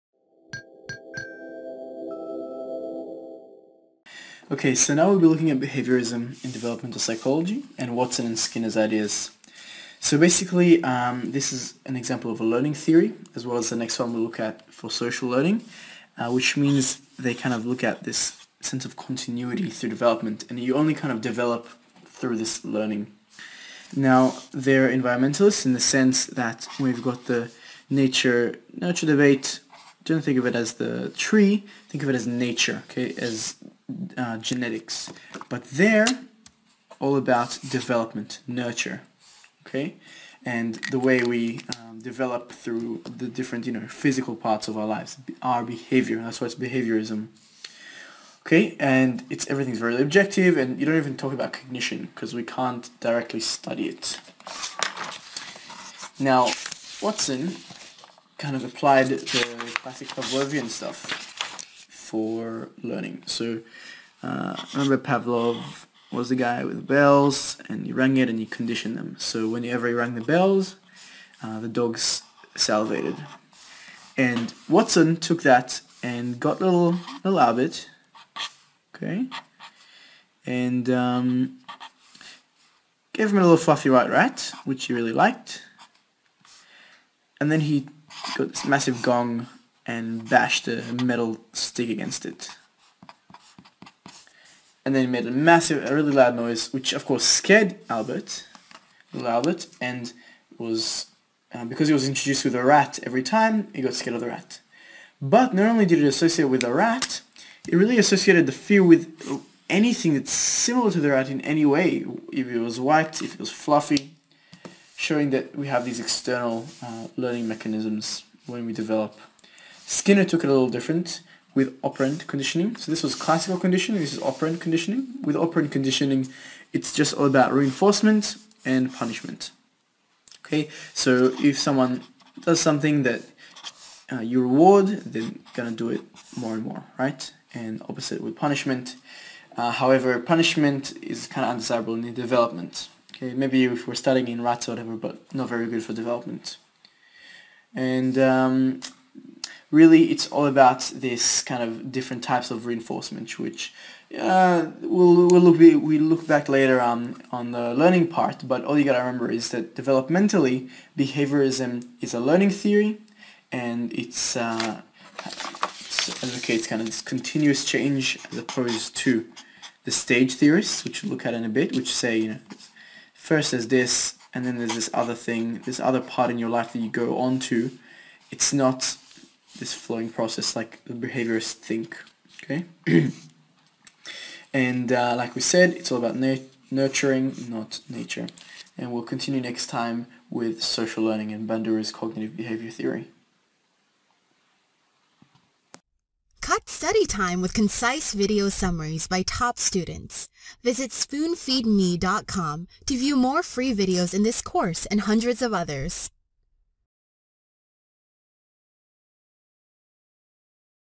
Behaviourism | Psychology (PSYC101)
for more free video tutorials covering Psychology.

This video briefly demonstrates behaviourism by explaining three different models subsequent to appropriate examples. First part of the video explains basic ideas of learning theory where it shows how environment shapes development of behavior and changes are quantitative rather than qualitative. It also focuses on objectivity following an example on avoidance of cognition.

The video, in its’ second part, explains Watson model applying classical pavlovian principles of learning to development. It introduces the studied conditioned emotions such as rage and fear giving an example of little boy Albert where it shows how associated fear with white fluff due to loud noise of rats can significantly affect Albert’s behaviorism.

Last part of the video describes Skinner model in details applying operant conditioning where it shows how behaviors shape through reinforcement or lack thereof to create strengthening or extinction. It also illustrates effectively of punishment and shows why it is so undesirable due to adverse consequences.